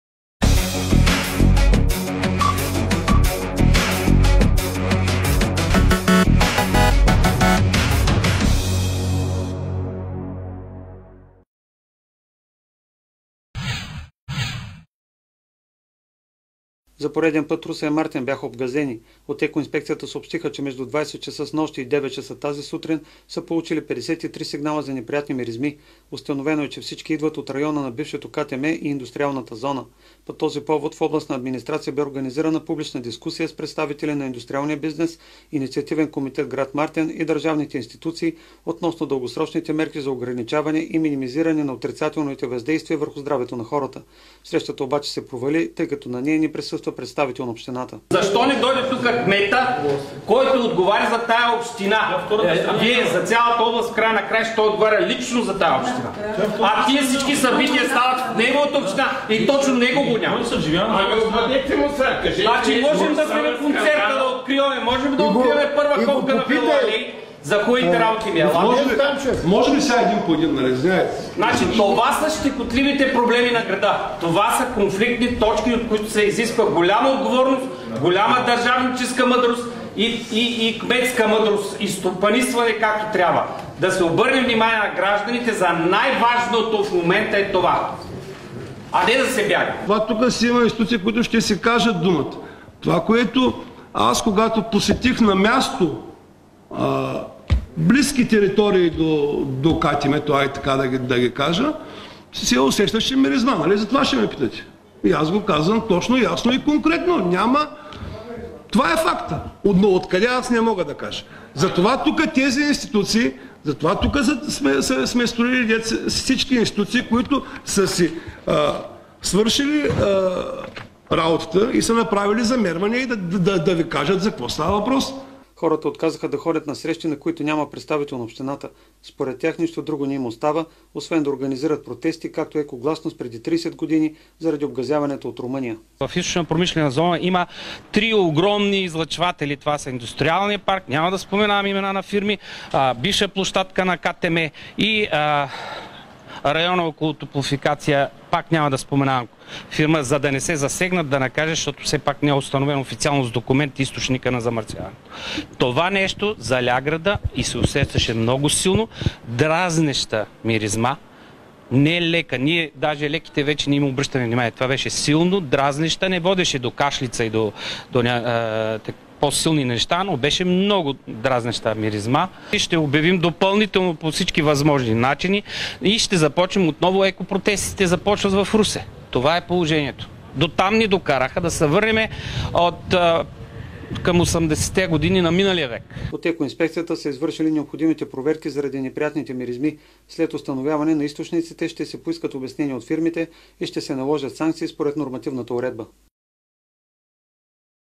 Русенци се заканиха да започнат протести както преди 30 години
Провали се срещата във връзка с обгазяванията на Русе и Мартен организирана днес от областна администрация. Причината за това стана отсъствието на представител на общината. Хората се заканиха да започнат протести както преди 30 години.